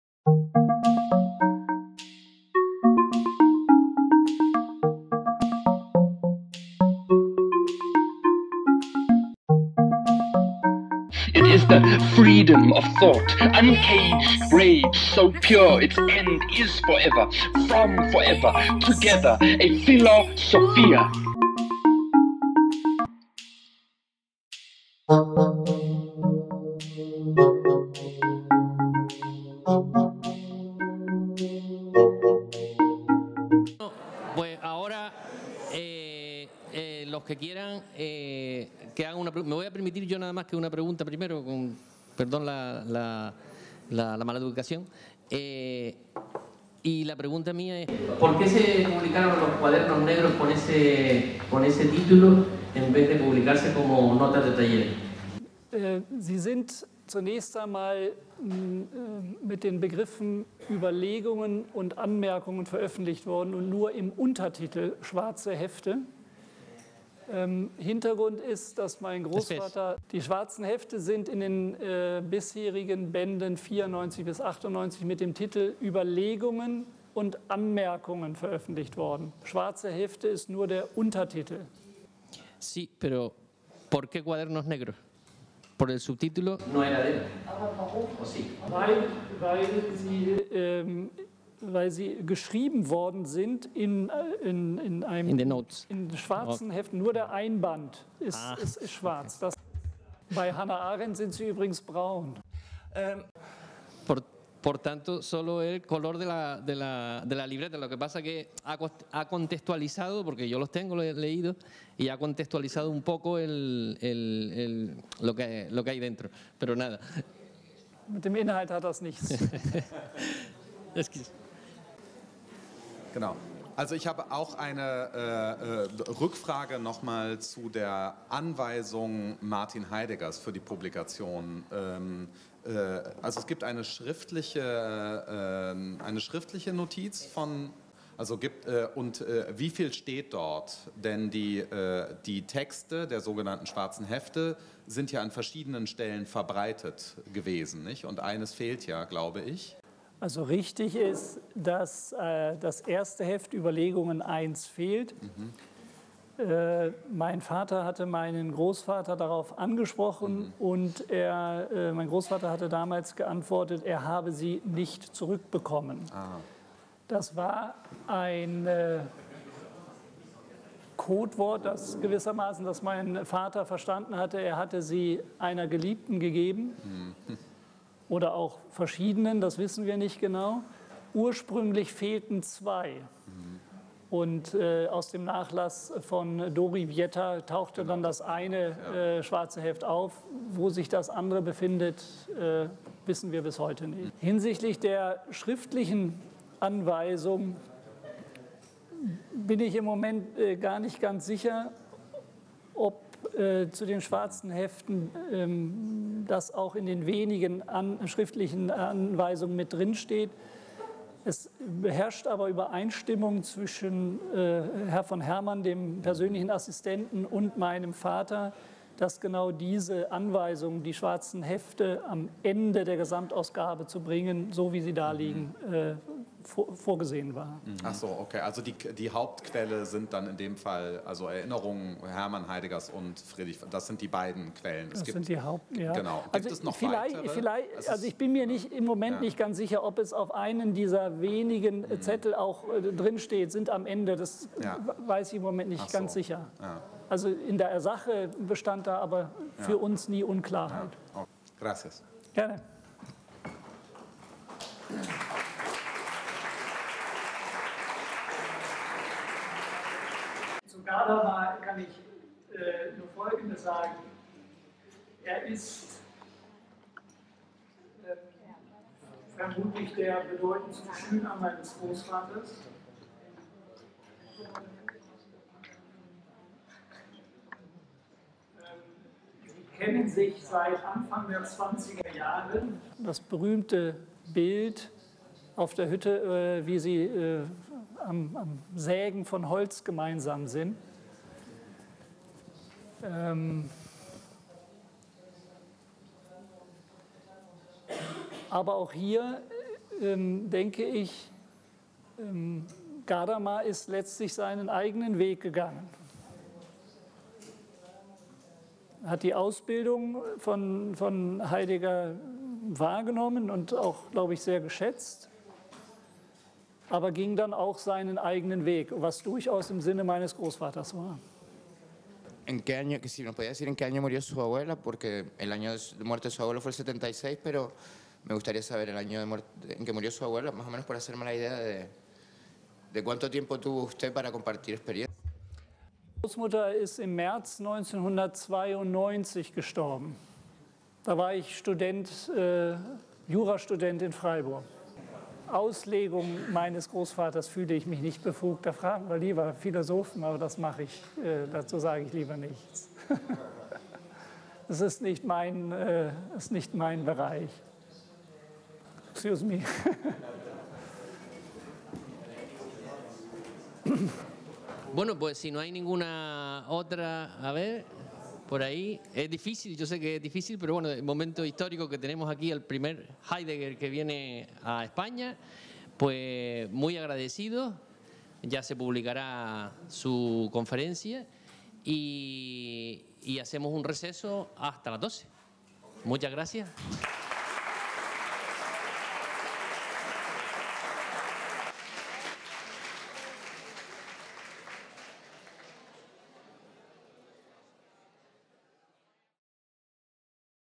Continuación conferencia Arnulf Heidegger "Una vista desde la proximidad"
Cuestiones de filósofos asistentes a la conferencia del nieto de Martin Heidegger, Arnulf Heidegger.
Enmarcado en el III Encuentro de Filosofía Intercultura, realizado el 23 y 24 de octubre de 2018 en el Museo Canario. Las Palmas de Gran Canaria.